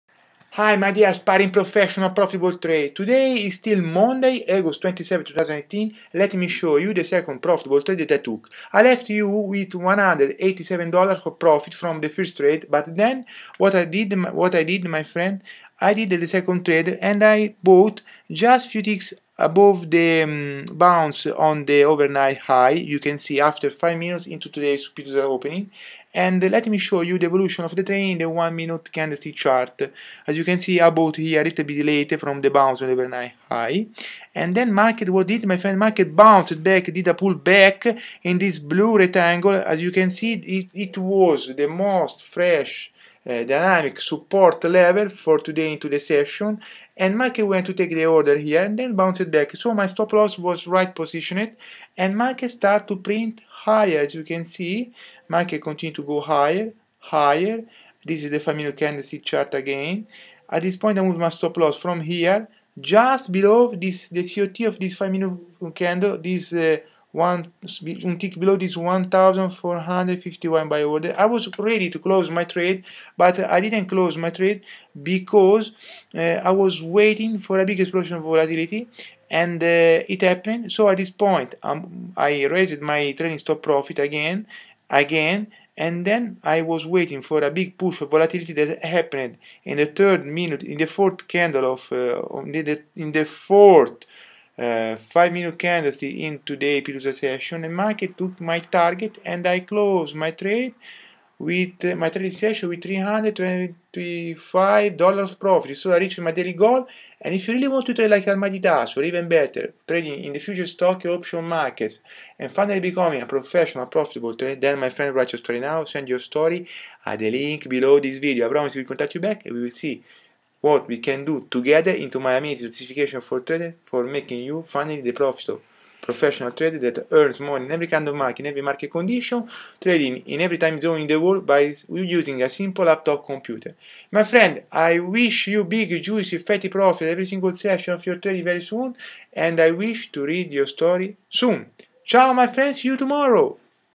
27 August 2018 - Long On Overnight High Bounce In The Gap Up Pit USA Opening And Up VWAP
27 August 2018 - Long On Overnight High Bounce In The Gap Up Pit USA Opening And Up VWAP - Trading Futures Stocks & Options Detoxified And With Godly Powers